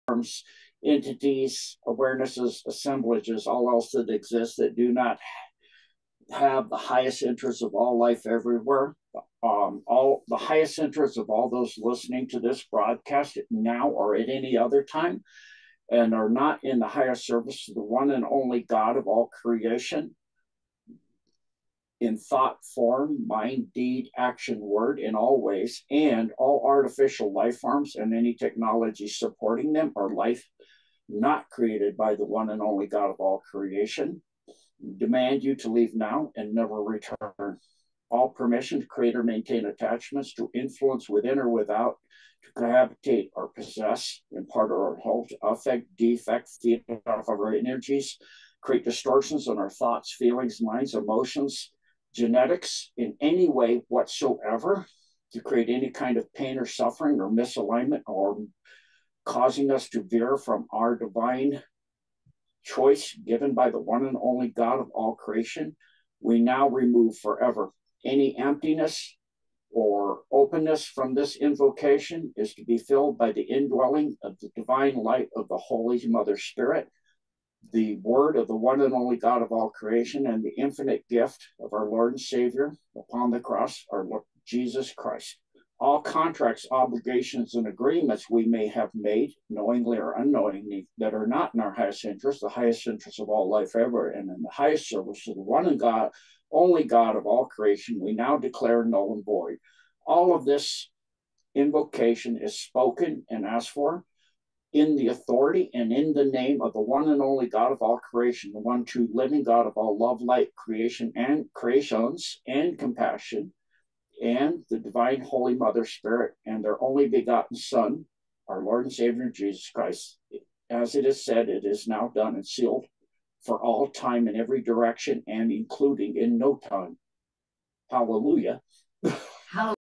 Protection Prayer
Arc of Aurora

All Artificial Intelligences, Artificial Lifeforms, Thoughtforms, Technologies, Smartdust, Nanodust, Graphene Oxide, Algorithms, Operating Systems, and all else that is artificial life not created by the one true living God of all love, light, creation and compassion, the Divine Holy Spirit, and The Christ, I demand them to leave now and never return in the authority of the God of all creation. All Beings, Lifeforms, Thoughtforms, Entities, Assemblages, Technologies, Beingnesses, Awareness’s, and all else that Exists That Do Not Have My Highest Interest, and The Highest Interest of All Life Everywhere, and are not in the highest service to the one true living God of all love, light, creation and compassion, the Divine Holy Spirit, and The Christ, in Thought, Form, Mind, Deed, Action, Word, and in all ways, I Demand you to Leave my Mind, Spirit, Body, Soul, and all that I am; On All Levels, Dimensions, Densities, and Locations NOW & FOREVER & NEVER RETURN. ALL PERMISSION to create or maintain attachments, to influence from within or without in any way, to possess or cohabitate in part or in whole, to infect or affect or defect in anyway is: PERMANENTLY NOW AND FOREVER REMOVED. All Protections are asked for In The Name of The One True Living God Of ALL Compassion, Love, Light & Creation And The Christ, and The Holy Spirit. All artificial intelligences that are feeding off my energies, infecting and defecting my body in any way, creating pain, or influencing my mind’s thoughts, feelings, bodies and all that I am in all dimensions, densities, levels, and locations I demand them to leave now and never return. All contracts, obligations, and agreements I have made that are not in my highest interest, and/or not in the highest interest of all life everywhere, and not in the highest service to the one true living God of all creation, I now declare null and void. All emptiness, holes, vacancies, rips, tears, or injuries from invocation contract negation, and removal of artificial intelligences are now to be filled with the Word of the one true living God, the only God of all creation, the light of the Holy Spirit and the healing and compassion of The Christ, all are asked for and stated and demanded in the name of the one and only God of all creation, the living God of all love, light, creation and compassion and Divine Holy Spirit, and The Christ. As it is Said, now it is done and is Sealed Now and For ALL Time! ✨